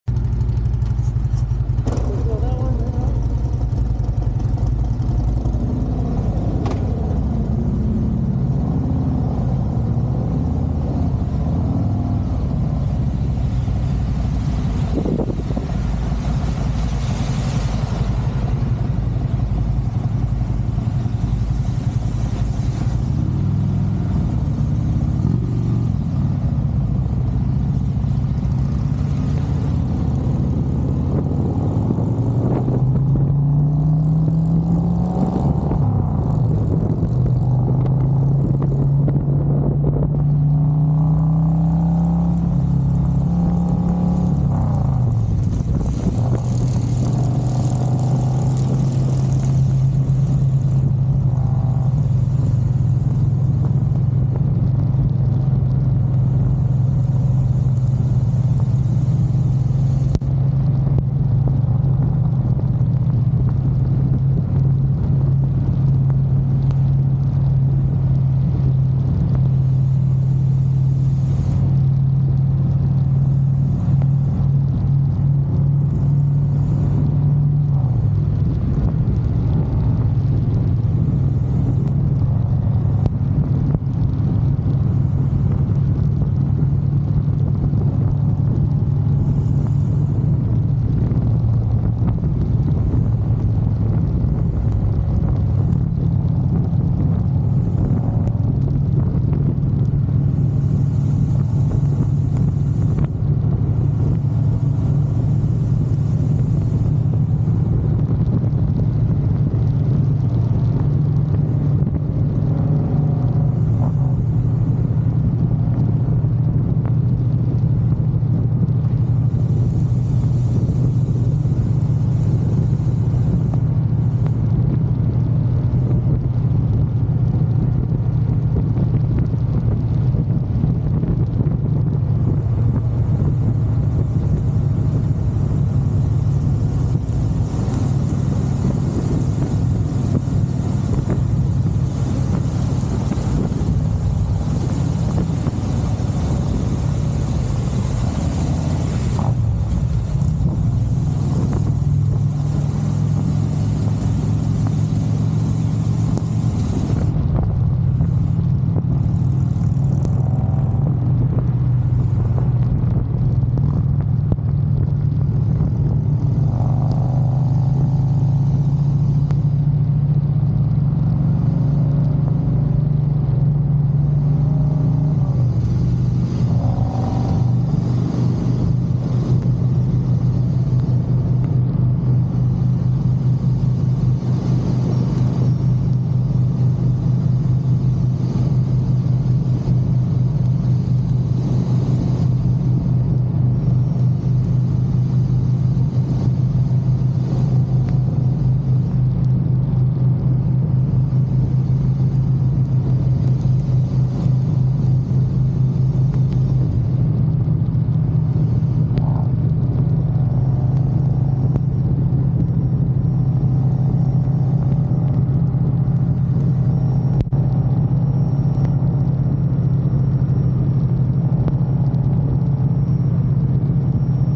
Riding in the Rain
We didn’t plan to ride in the rain but it happens.